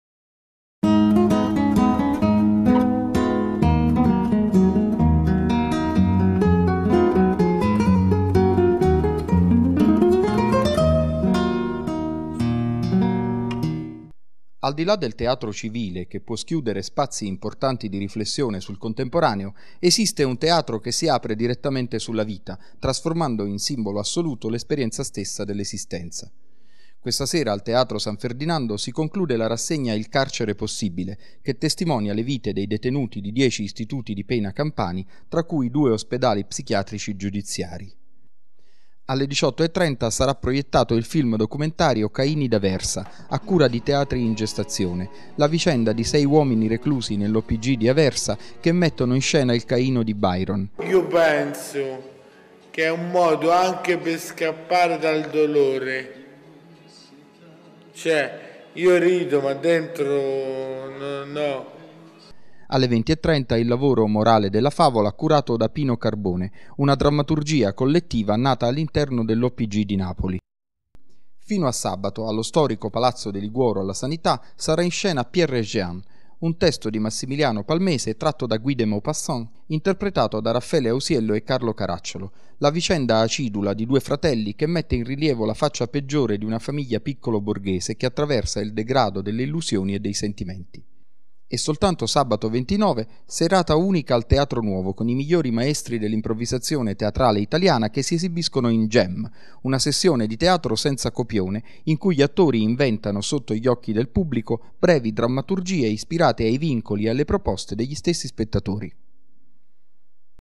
Indovina chi viene in scena (28 ott 2011)
Proposte per il teatro nel weekend a Napoli (28-30 ottobre 2011).